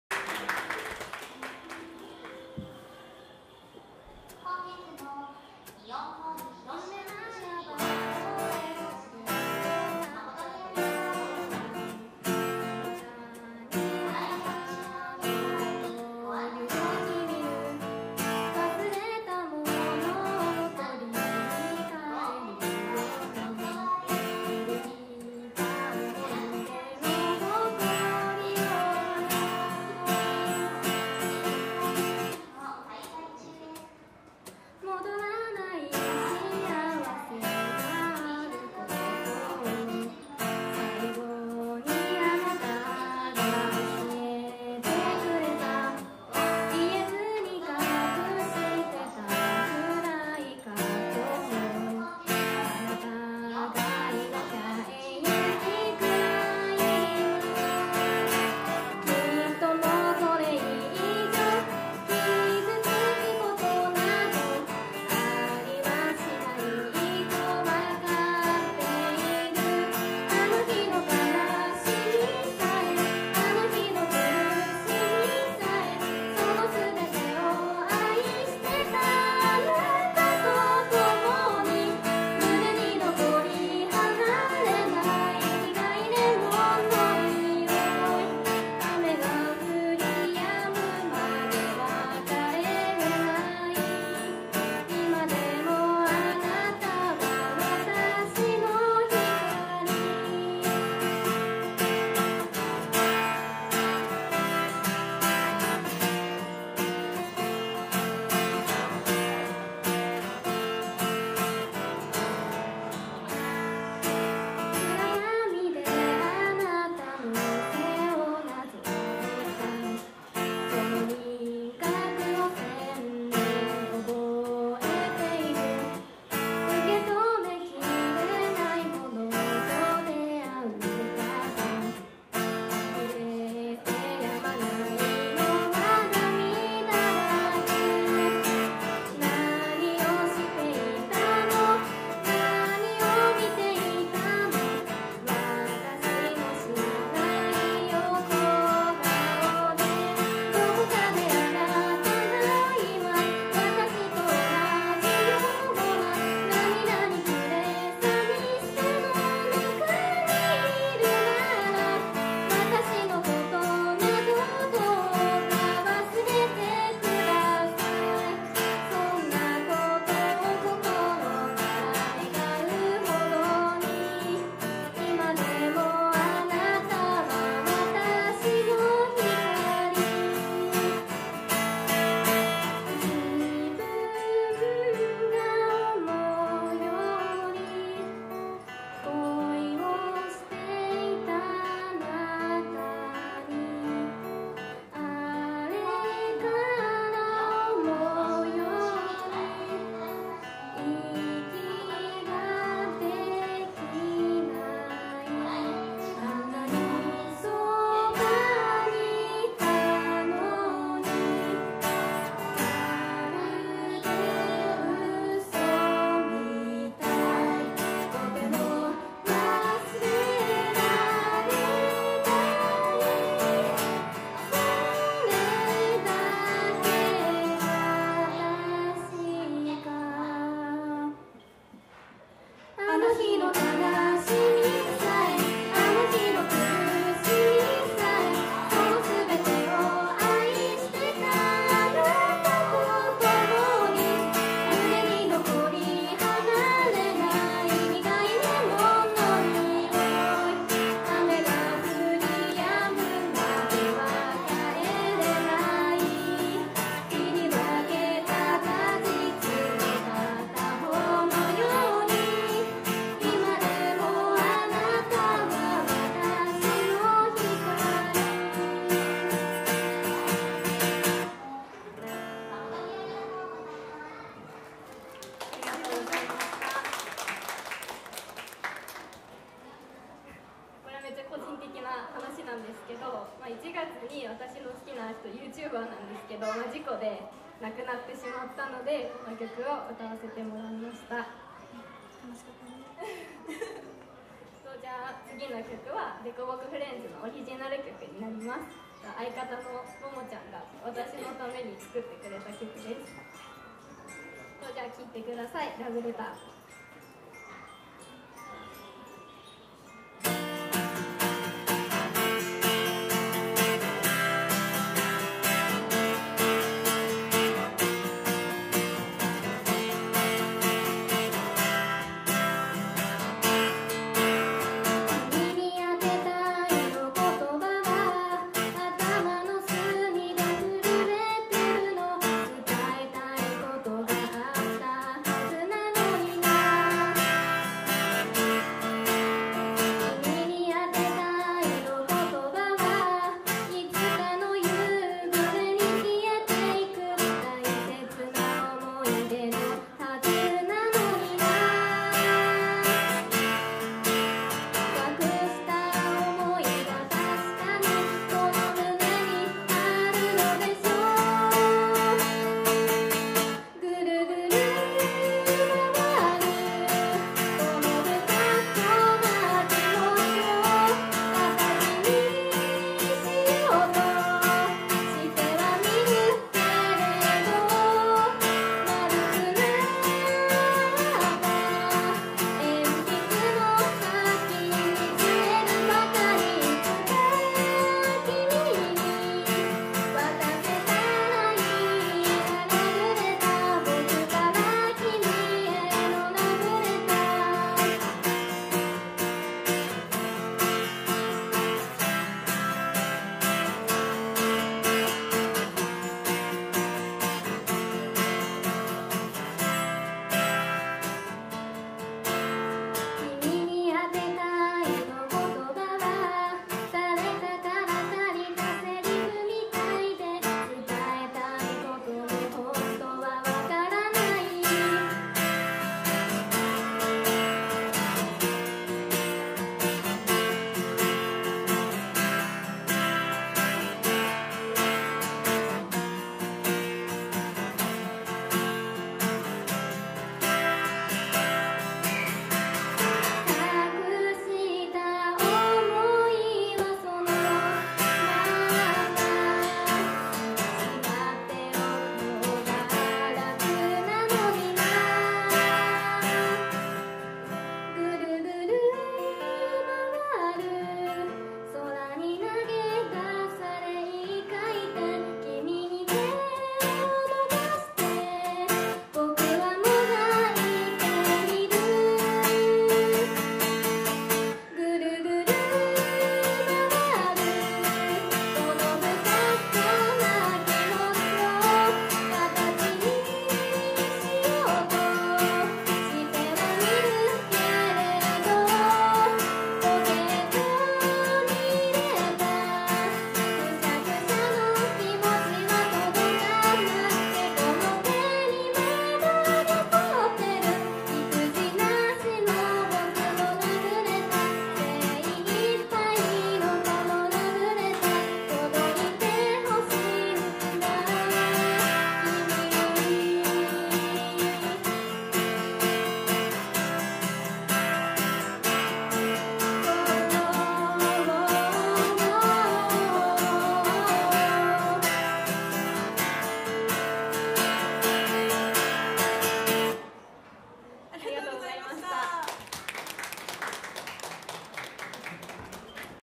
★第5回アコパラ「凸凹フレンズ」
2019/3/16に行われた第5回アコパラ府中店店大会での演奏です。